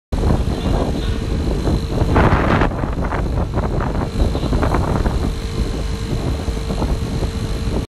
EV850 laserscreed
Automatic small laser screed machine make your concrete much better